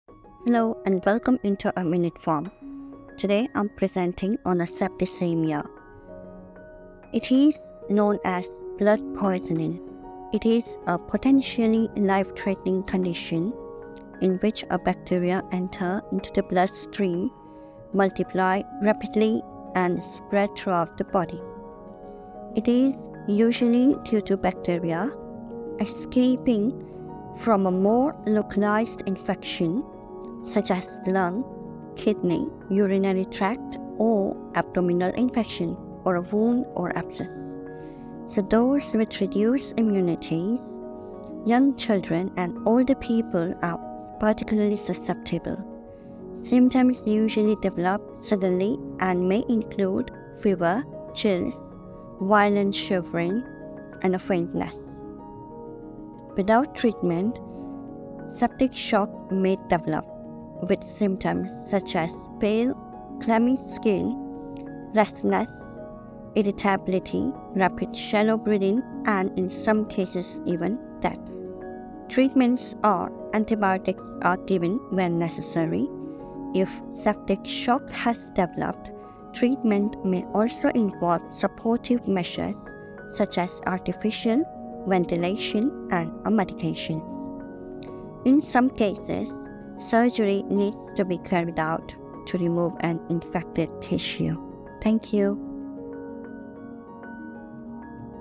Septicaemia/ Blood poisoning
what is Septicaemia?
how serious is it?
what are hidden symptoms?
treatment options?